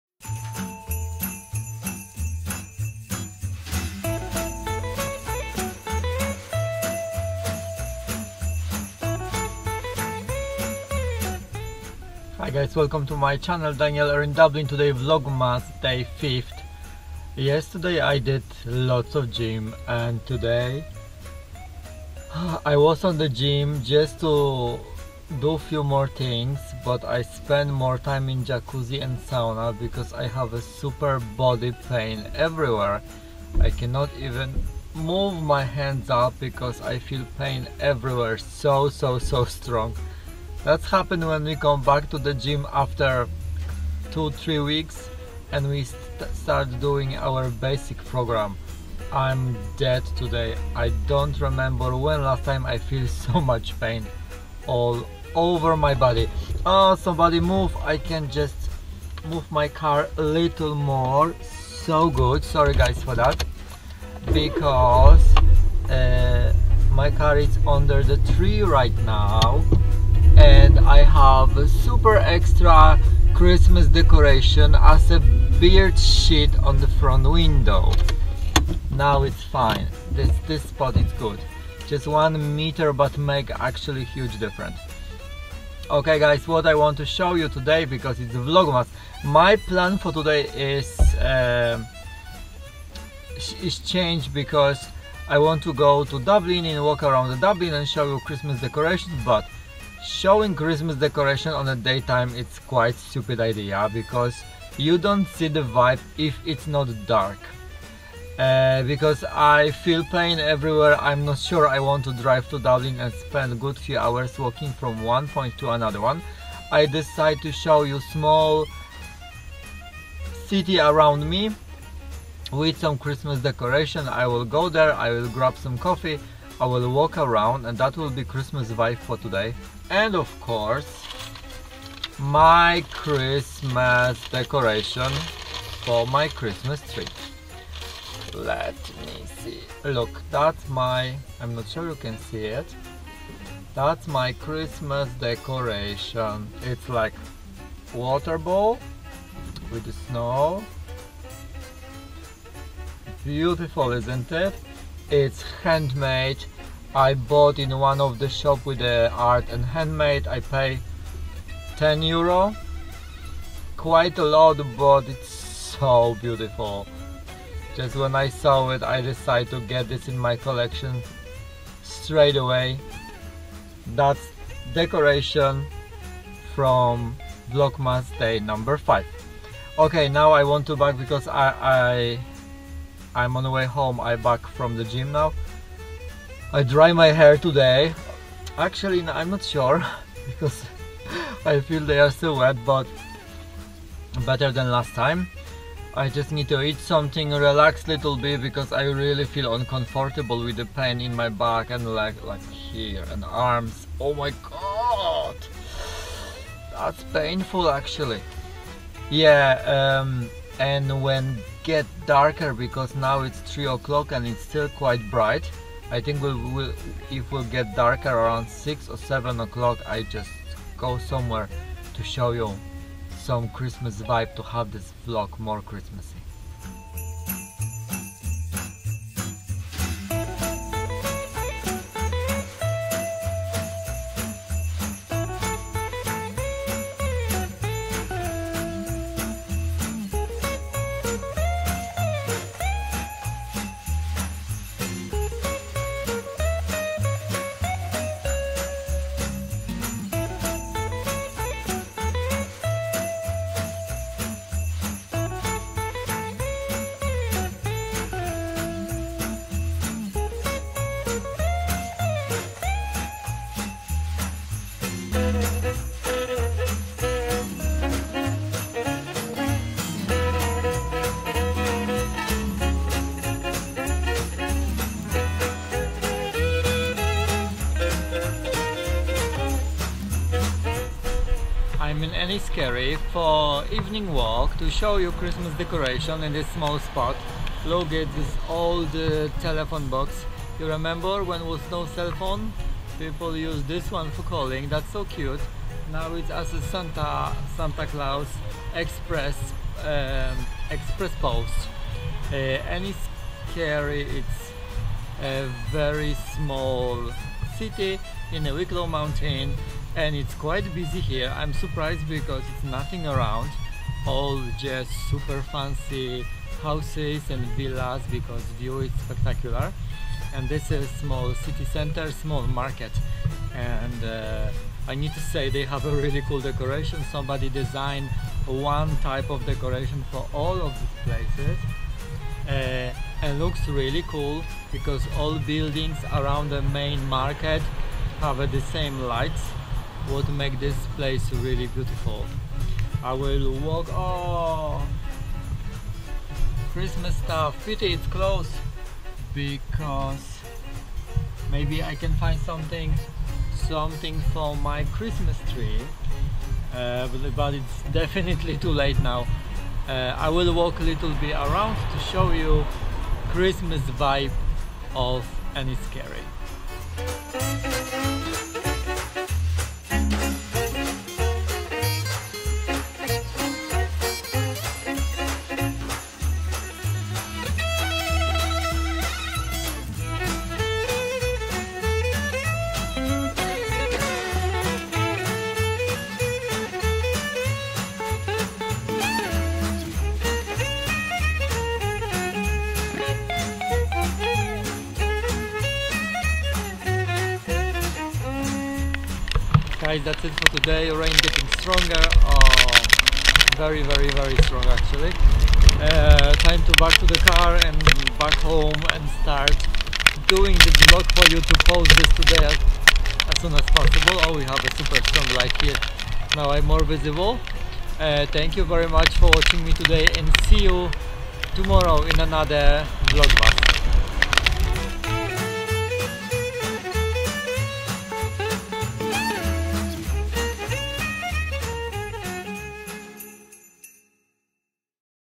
Vlogmas #5 night walk in Eniscorty
Vlogmas 5

I bot one of the most beautiful christmas decoration ever.
Night walk in Eniscorty